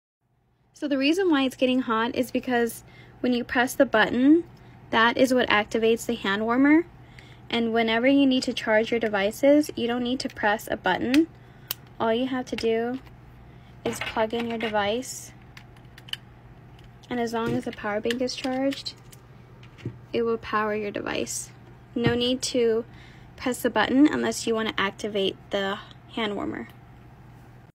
Reply to @brandybugatti when you charge your devices just plug it into the power bank!
* Contains lithium battery - can only be shipped to the US domestically (No international shipments, sorry!)

Cute AF! Is this a Power Bank? AND a Hand Warmer?!

How many times have you been out and needed a charge? Impossible to count, right? This fun traveler power bank has your back when your phone decides to die right as you leave the house. Plus, with a built-in hand warmer, you can warm up frozen fingers in a flash. Take this power bank hand warmer with you every time you need some extra battery life and warmth.

Large 5500mAh battery capacity can fully charge up a completely dead phone (and more)
Doubles as a hand-warmer to keep your fingers warm
Compact and lightweight (and even shaped as a suitcase). Perfect for on-the-go!